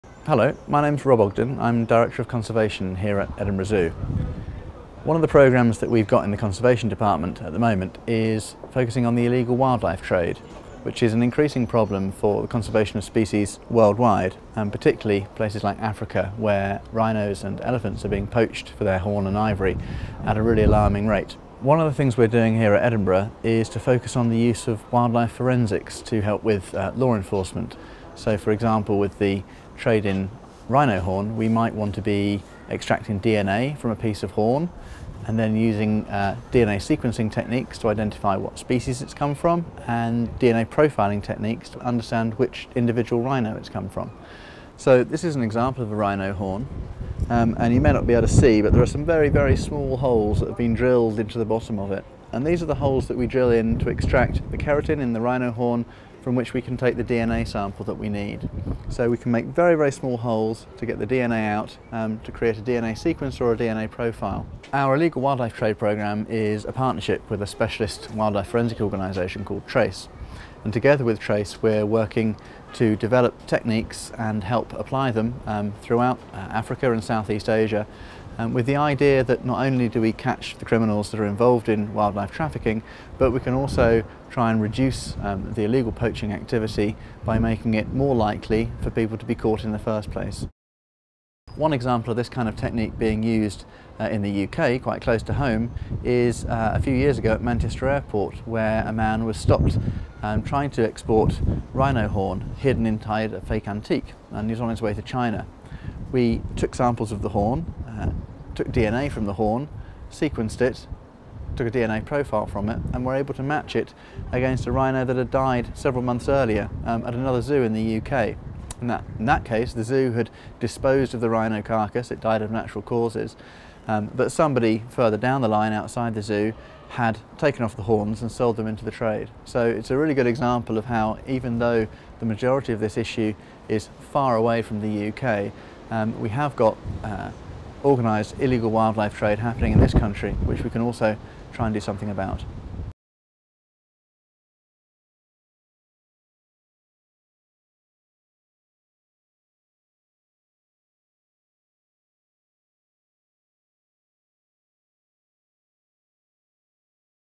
Illegal Wildlife Trade - RZSS Conservation Projects - Great days out for a good cause
The illegal wildlife trade is an increasing problem for the conservation of species worldwide. Species such as rhinos for example, are being poached for their horns at an alarming rate.
The RZSS has partnered with a specialist forensics organization called TRACE. Together we are using wildlife forensics to assist with the law enforcement. Through DNA sequencing and profiling it is possible to identify both the species, and even the individual, from which a piece of horn or ivory has come from. Through more effective law enforcement it is hoped that not only will more criminals be caught but that will also act as an increasing deterrent to future poaching activities.